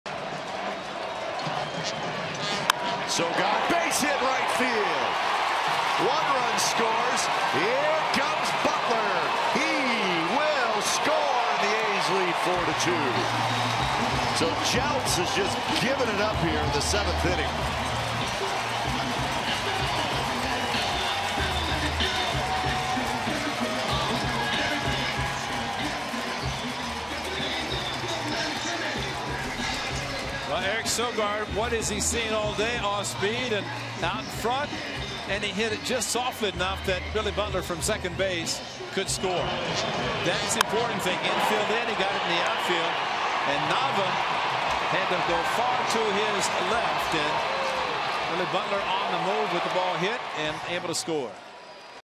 TB@OAK: Sogard singles in two, puts A's ahead in 7th
8/23/15: Eric Sogard singles to right field to score two runs and give the Athletics a 4-2 lead in the 7th inning